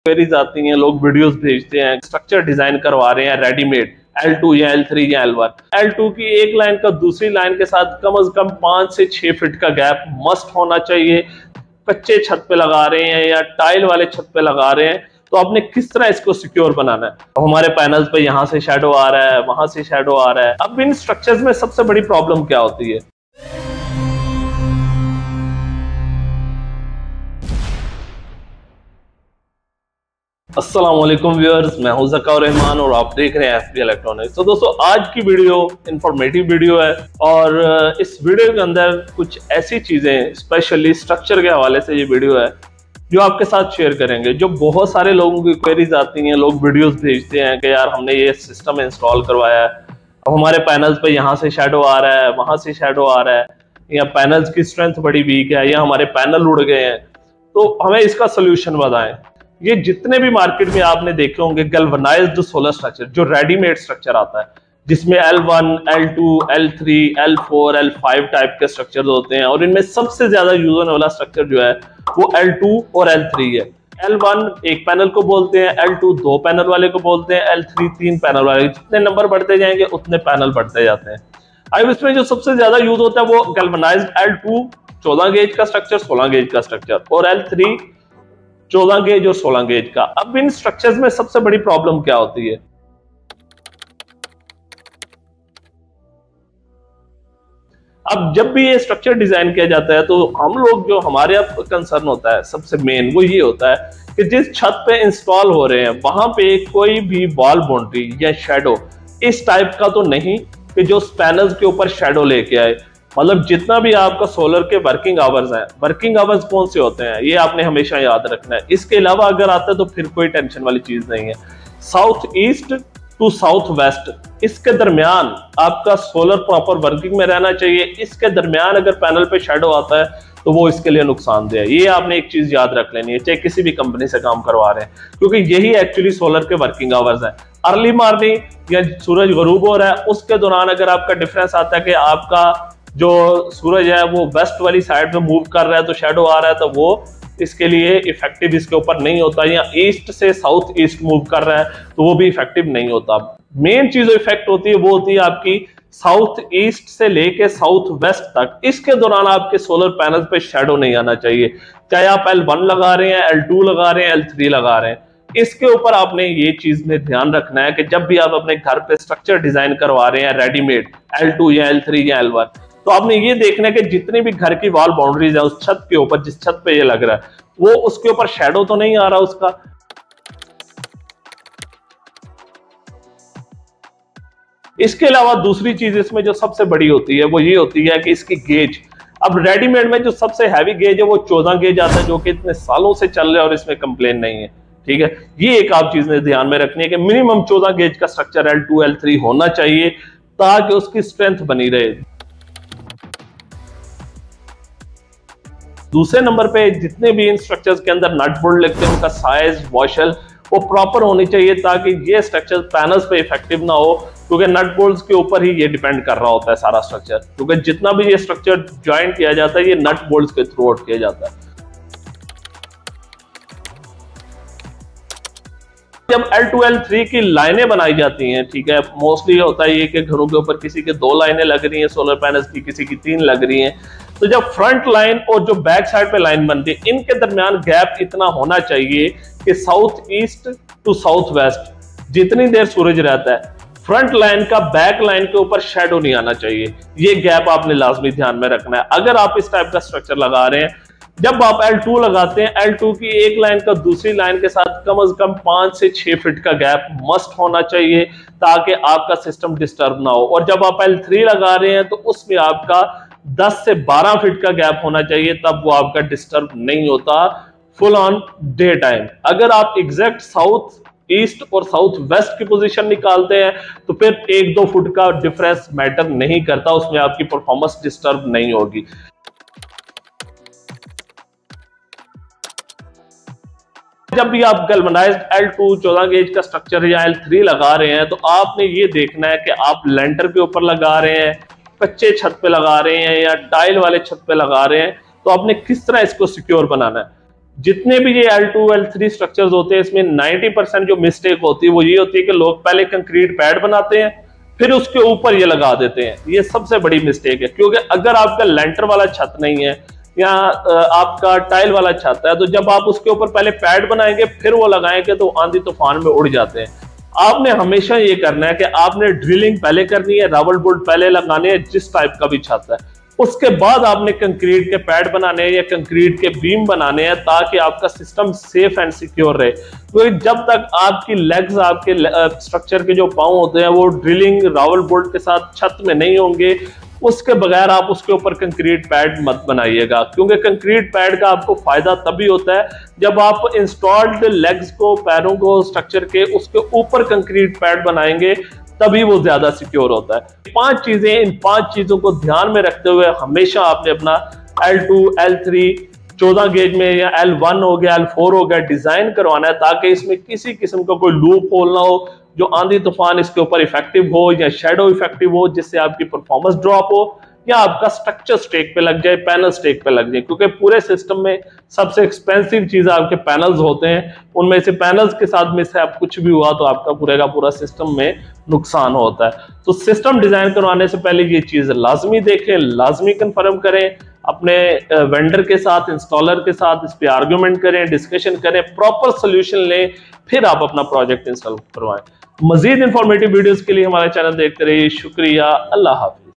Major Mistakes in Designing Galvanized L2 L3 Structures
"Discover the crucial errors to avoid in the installation of solar panels on galvanized L2 L3 structures. Uncover the major mistakes that could compromise your solar energy system's efficiency and longevity. Learn how to optimize your design for maximum performance and durability."

"Such informative videos offer invaluable guidance for the installation of solar systems."